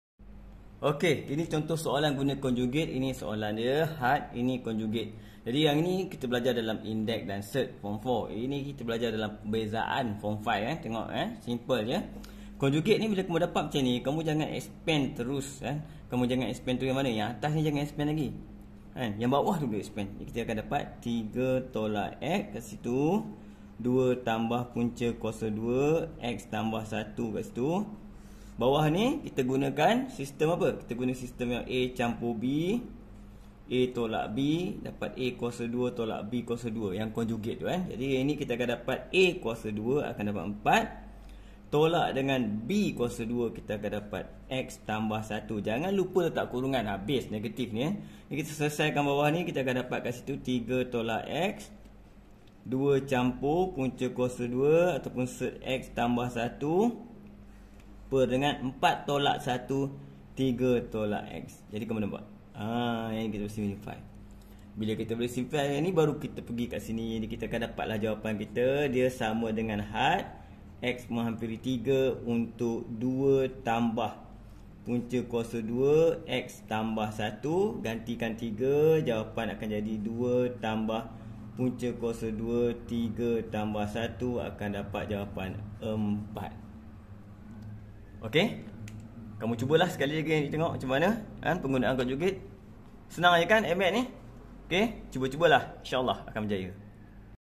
Addmath Pembezaan f5 teori had : Tanya wasap CG man
soalan ni ditanya oleh student melalui wsap